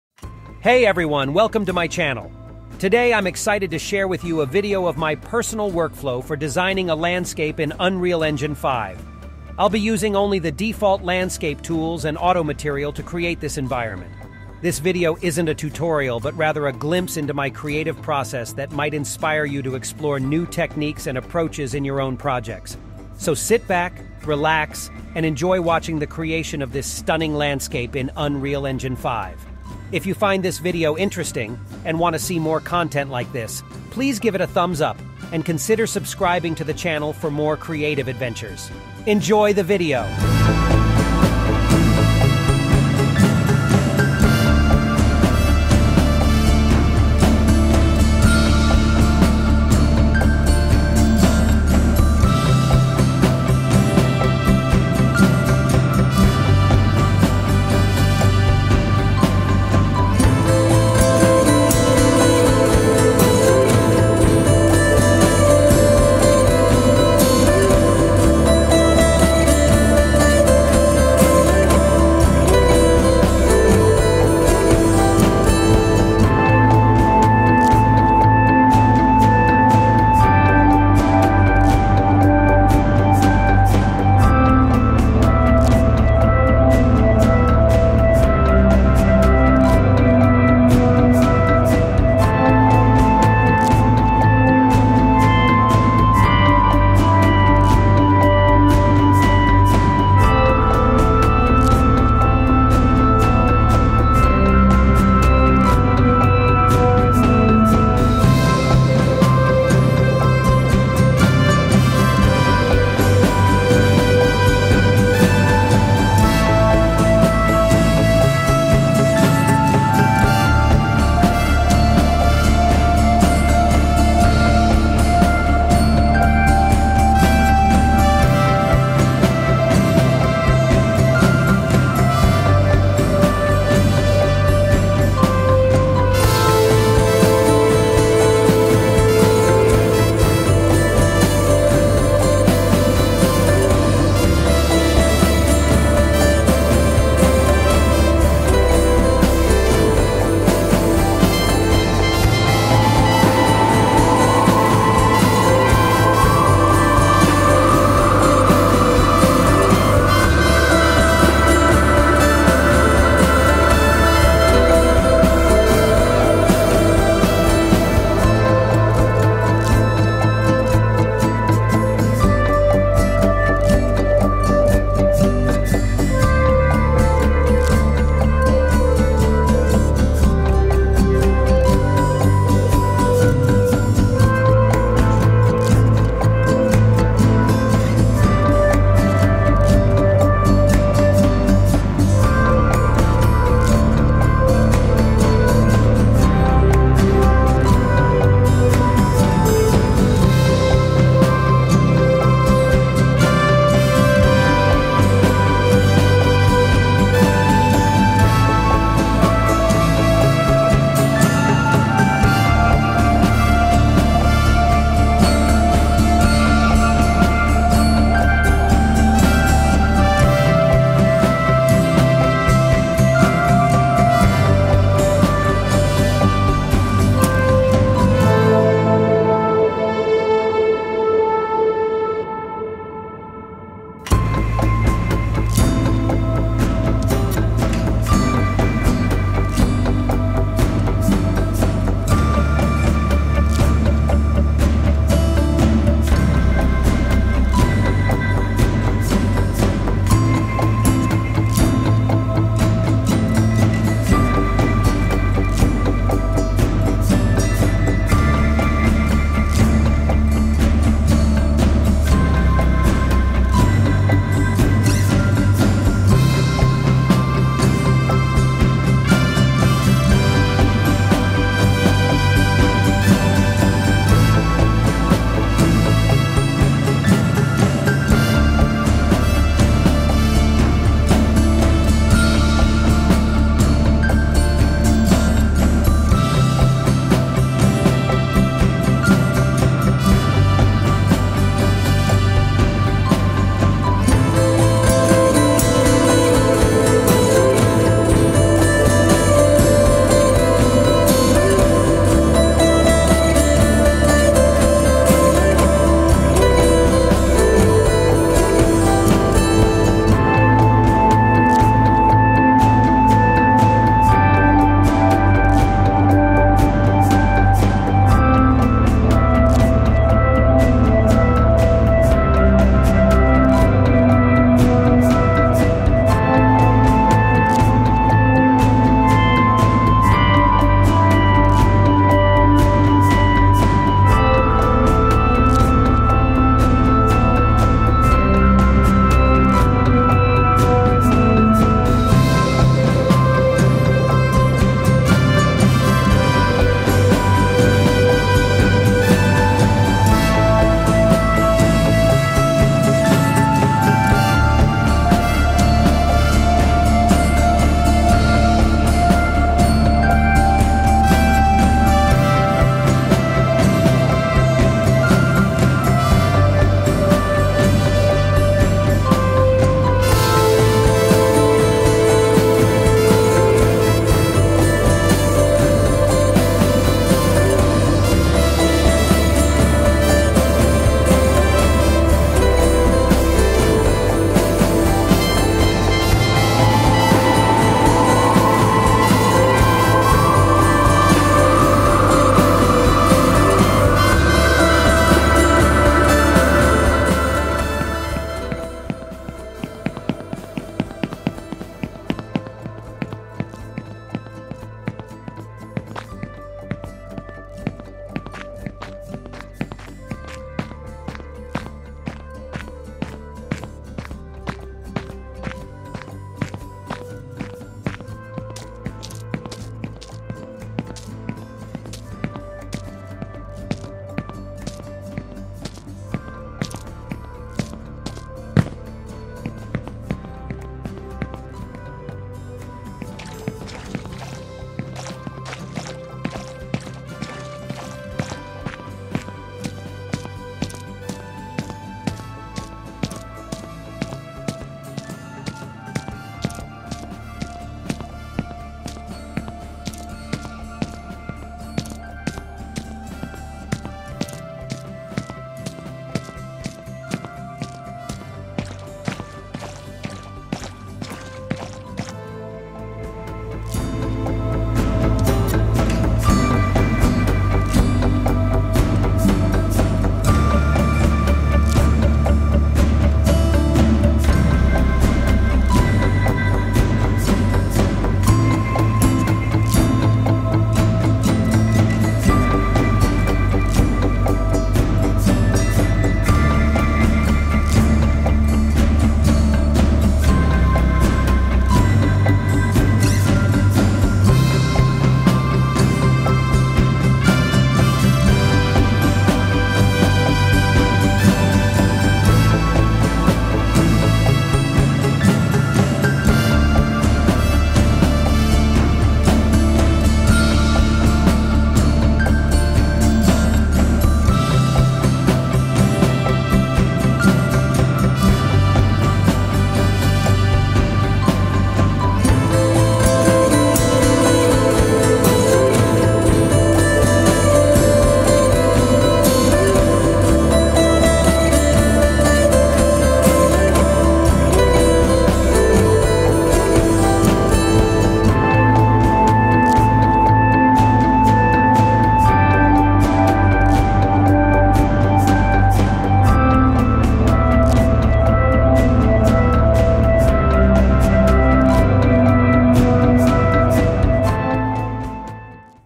UE5 Workflow - Landscape With Auto Material
Explore my personal workflow for designing an eye-catching landscape in Unreal Engine 5, using only default tools and auto material. Get inspired by this creative process and discover new possibilities for your own projects. Enjoy the video!

00:00 - Adding Landscape
00:20 - Sculpting the Landscape
03:20 - Adding Auto Material
04:15 - Adjusting Foliage
05:18 - Adjusting Material Settings
06:37 - Final Results